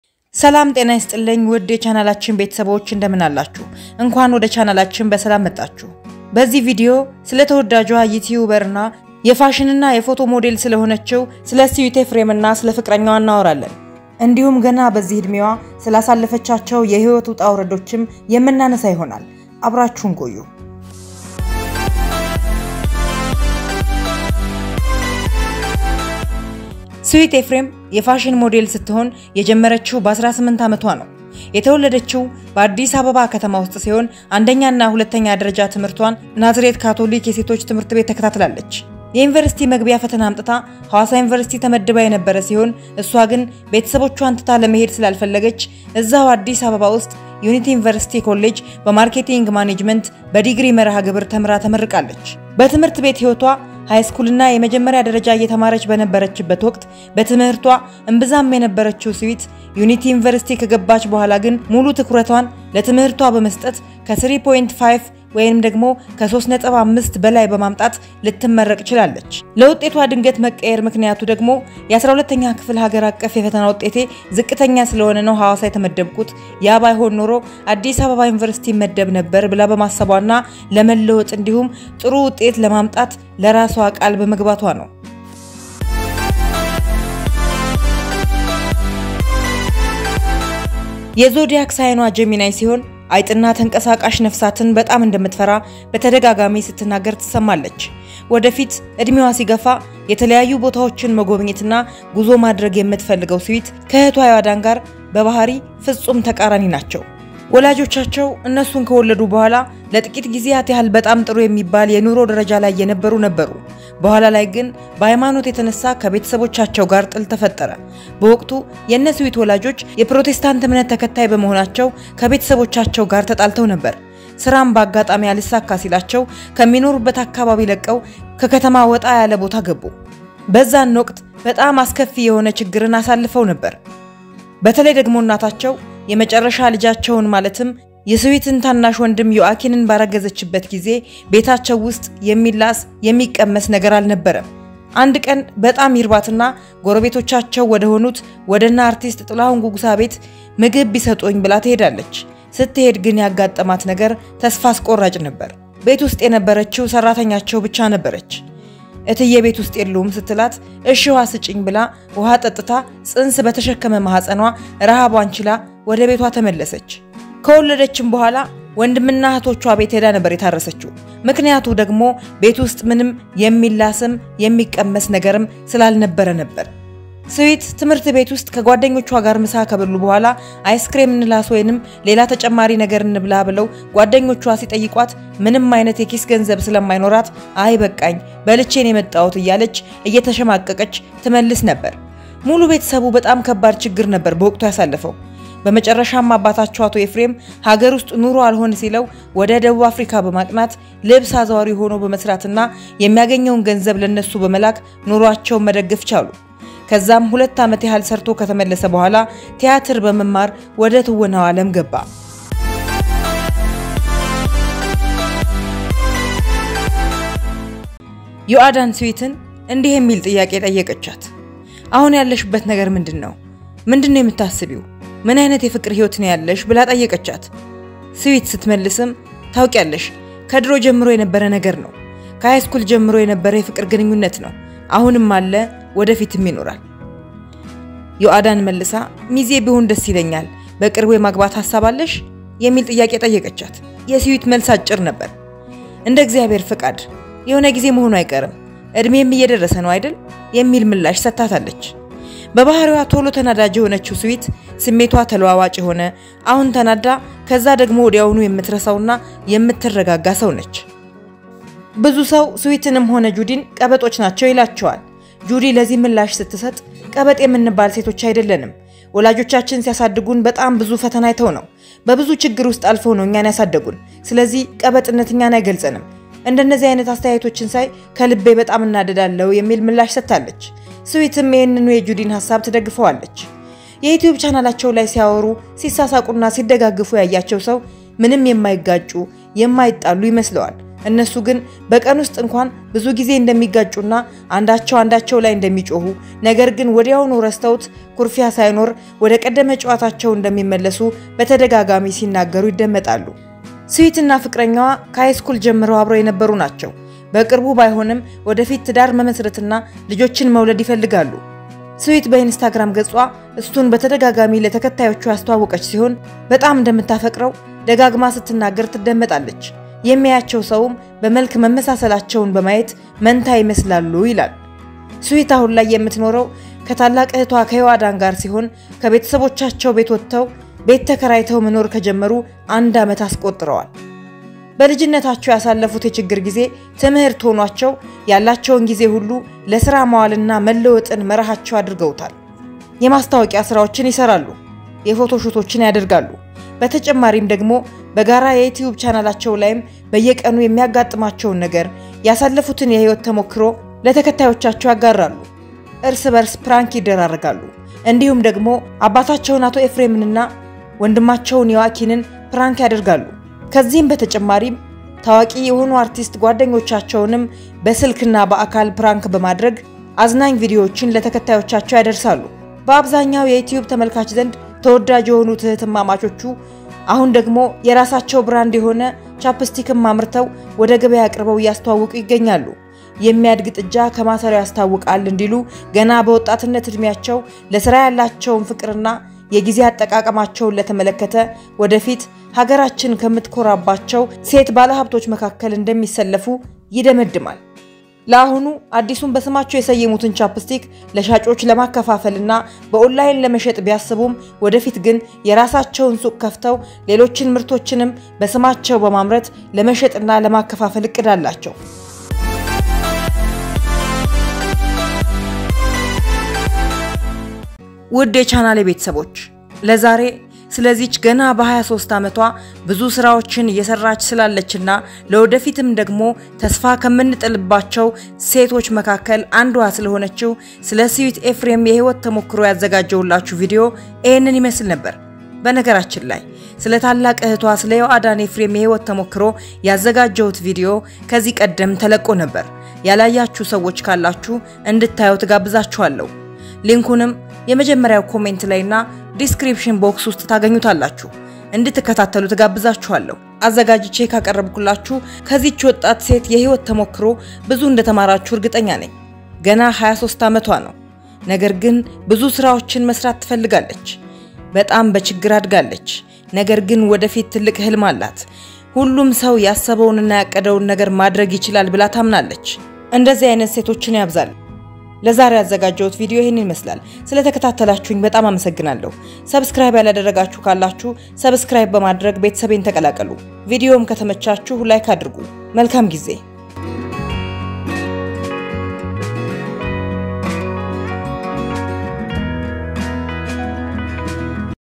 የስዊት ኤፍሬም እጮኛ ማን ነው ??እንዴት ተገናኙ??❤❤❤/sweet ephrem /jehoaddan#/ጁዲና ስዊት /ዮአዳንኤፍሬም
የዮአዳን የልጇ አባት ማን ነው? ለምን ተፋቱ?... ስለ እሷ የማናውቃቸው ነገሮች